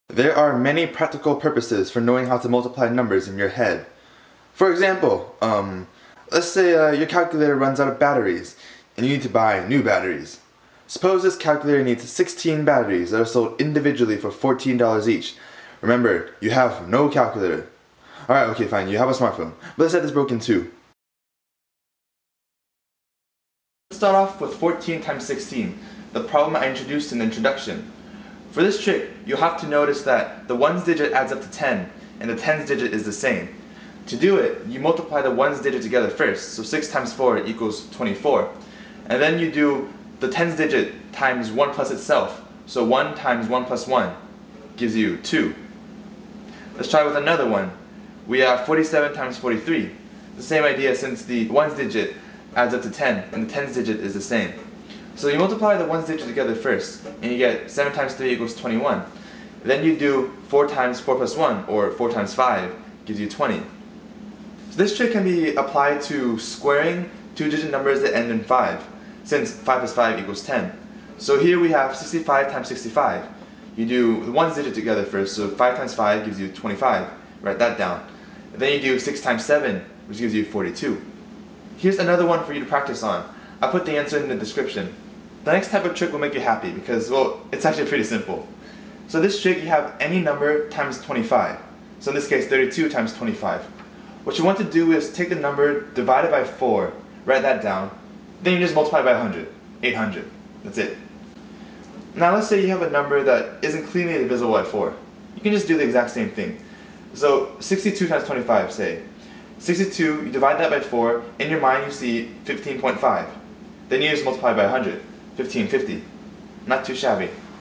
Multiply 2 digit numbers IN YOUR HEAD: simple scenarios
From the video: 82 x 88 = 7216
Tricks covered are AB x AC where B + C = 10 (such as 27 x 23), and AB x 25 (such as 32 x 25).
In case anyone is interested, here is a trick that works for all two digit numbers:
What you want to do is backwards foiling (or LIOFing) It works like this:
Say you have 22 x 37
do LIOF, or Lasts, Insides, Outsides, Fronts
Lasts: 2x7 = 14.  Write down the 4, carry the 1 in your head
Then do Insides plus Outsides: 2x3 + 2x7 = 20. Add the carried number and you get 21. So write down the 1 (to the left of the 4 you wrote down), carry the 2 in your head.
Fronts: 2x3 = 6. Add the carried number and you get 8.  Write that down, and you get the answer as 22 x 37 = 814